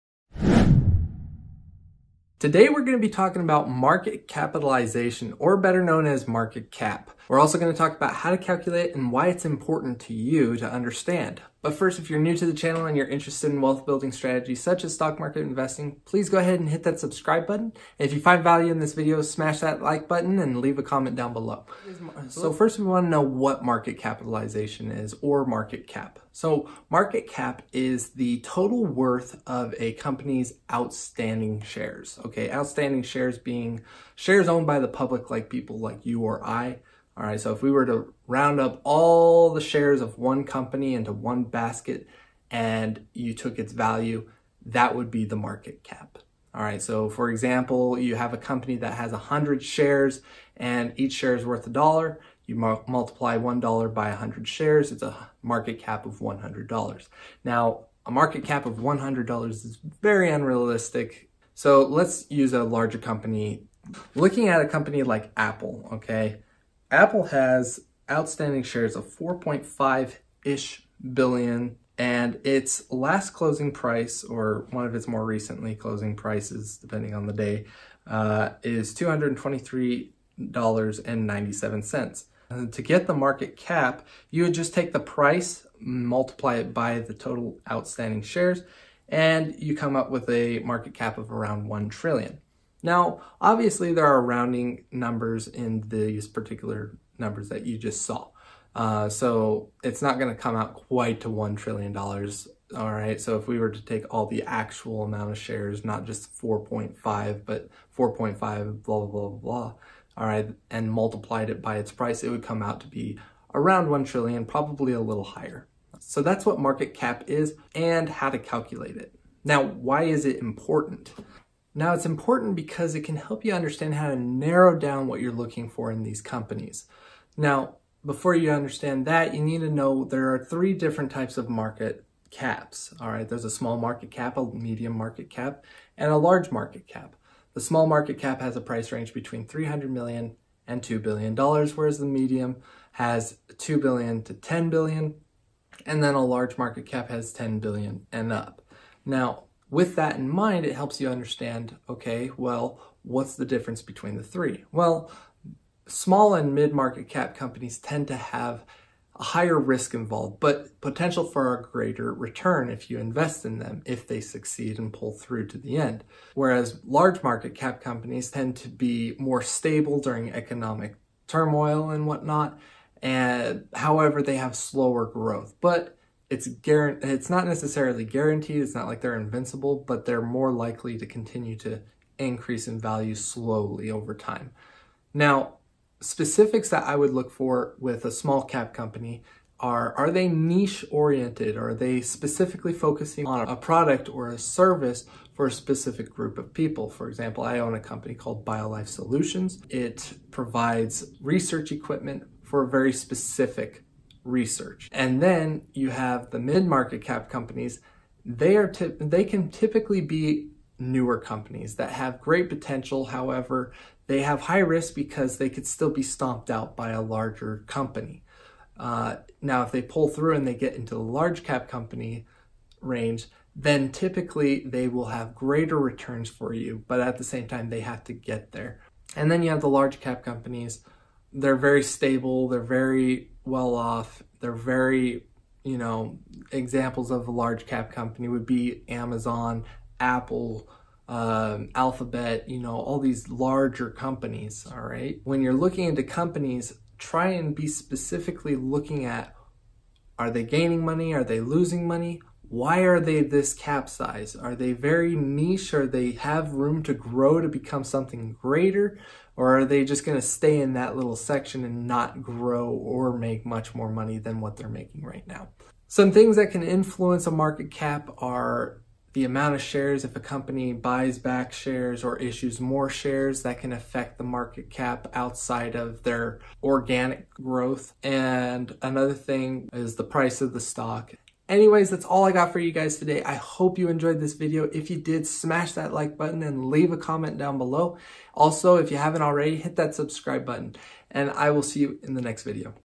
What is Market Cap? | How is market Cap Important? | How is Market Cap Found?
What is Market Capitalization? It is the worth of all of the total outstanding shares (shares owned by the public).

The market capitalization formula is calculated by taking the price of the stock and multiplying it by the number of outstanding shares.

There are three levels of market cap:

Small-cap: worth $300 Milion to $2 billion
Model-cap: worth $2 billion to $10 billion
Large-cap: worth $10 billion and up

How is market cap important to you as an investor? The following are ways you can use it in researching a company.

Things to keep in mind when looking at the market cap.

-Is it a niche company?
-Is it a new company?
-Does it have room to grow to a mid or large-cap company?
-Is it profitable or losing money?
-Is it stable?

Basic things that affect the market cap are:

-How many shares
-Price of each share

Hope this helps Wealth Giants,

Ryan

Intro Credit to ONdraid from panzoid